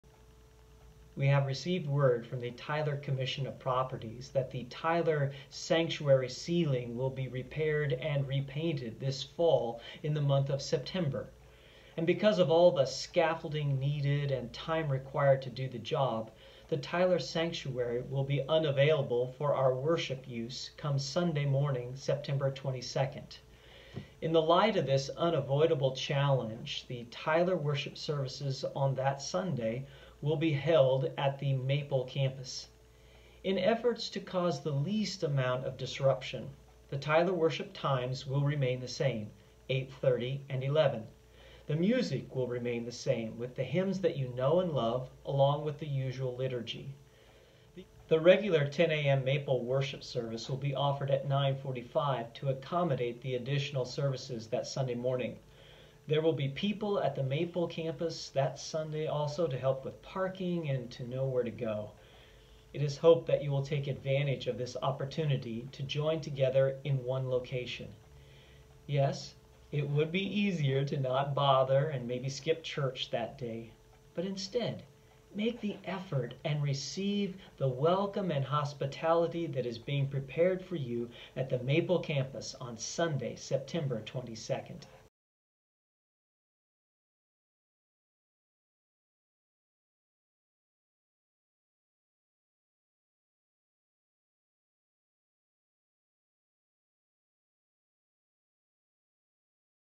Sept 22 Services Times and Locations
Sunday, September 22 all services will be held at the 12885 W. Maple campus.
8:30am Traditional
9:45am Contemporary
11:00am Traditional